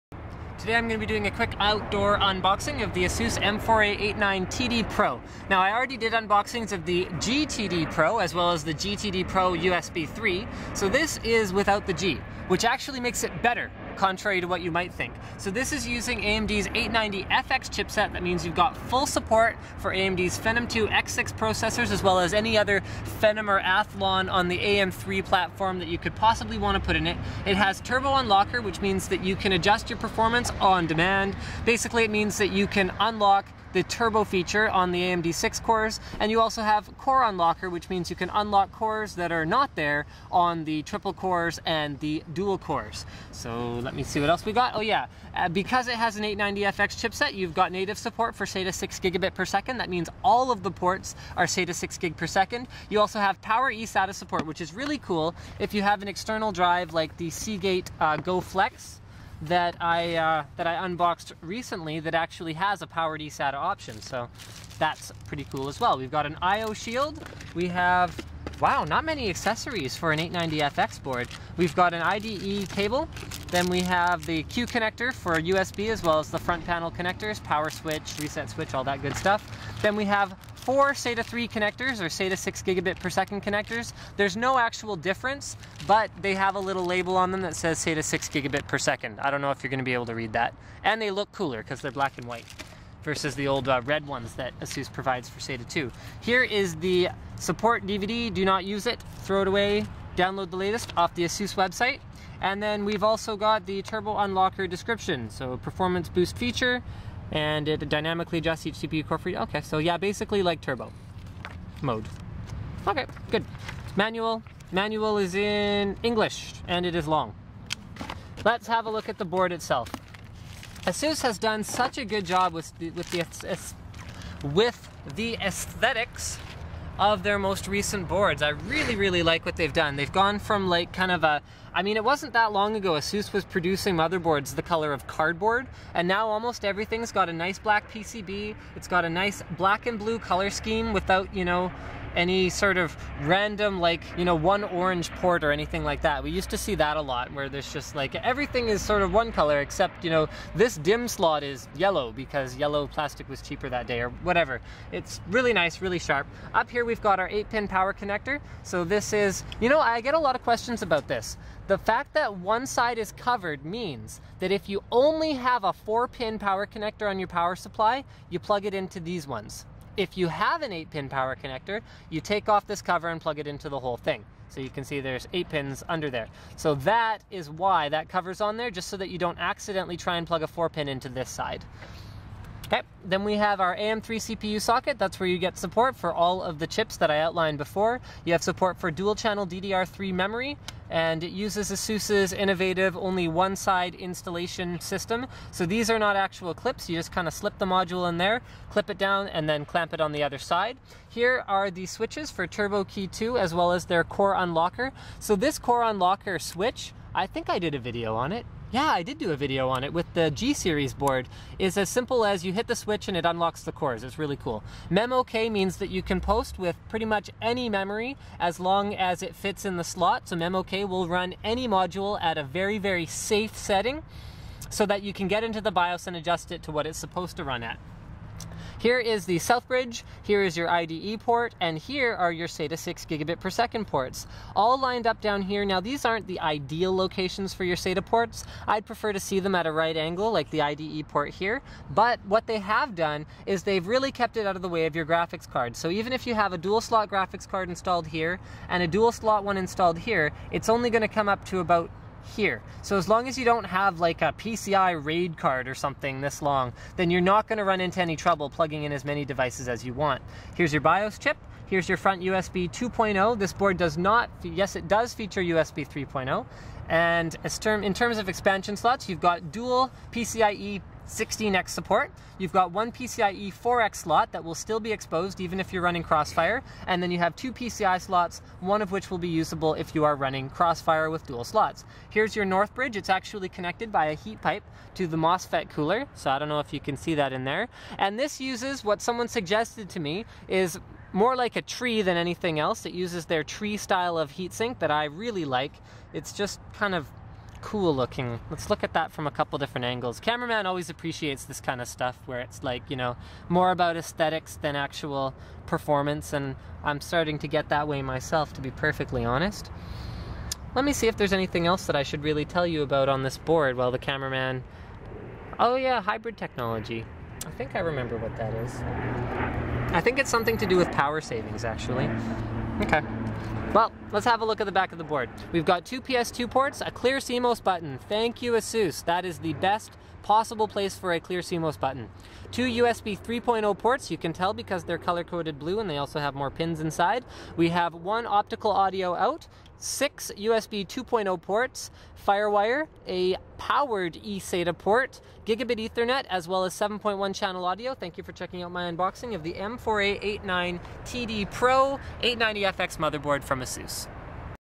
ASUS M4A89TD PRO 890FX SATA3 USB3 Crossfire Motherboard Unboxing & First Look Linus Tech Tips
Featuring support for the Phenom II X6 family from AMD, this is a board I've been meaning to unbox for over a month. Now I can finally do something else with it :P